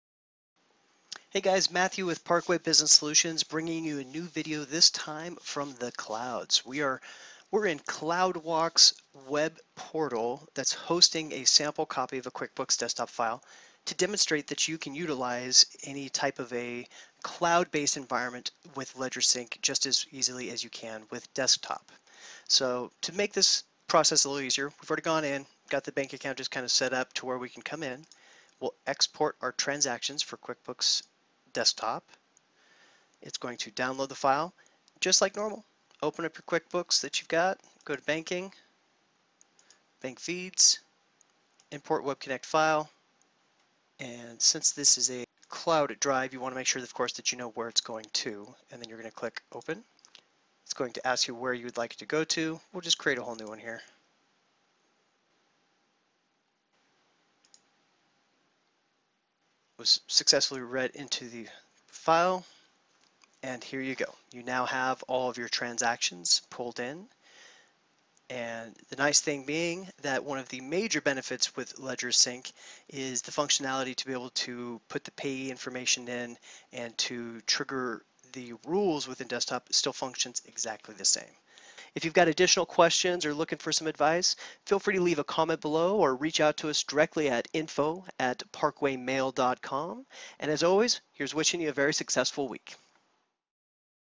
Cloud Hosted QuickBooks Desktop & Ledgersync
Even on a Cloud Based environment like Cloud Walk, Ledgersync  still reduces your data entry time significantly. This QuickBooks Data Entry video demonstrates how to import your banking transactions with Ledgersync.